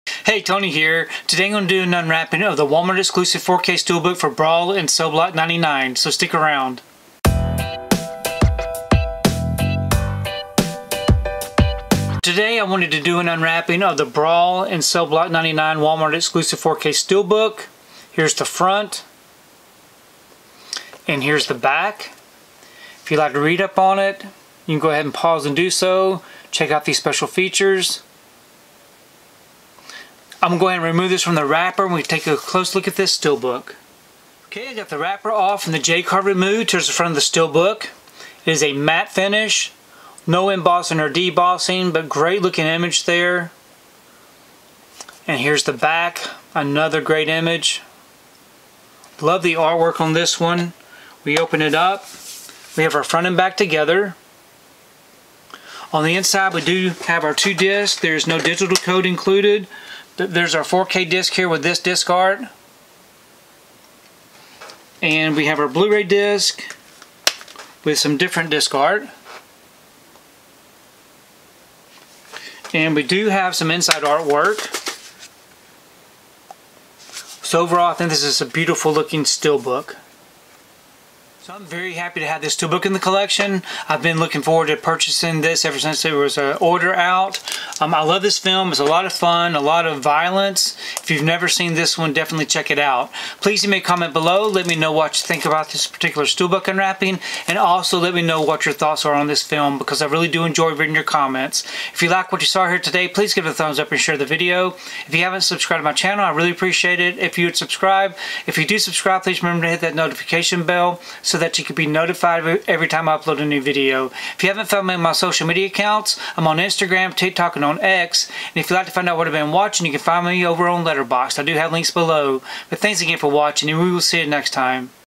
Brawl In Cell Block 99 4K Ultra HD Blu-ray Walmart Exclusive Steelbook
Check out my unwrapping of the Brawl In Cell Block 99 4K Ultra HD Blu-ray Walmart Exclusive Steelbook! @walmart

► Purchase the 4K Ultra HD Blu-ray
► Purchase the Blu-ray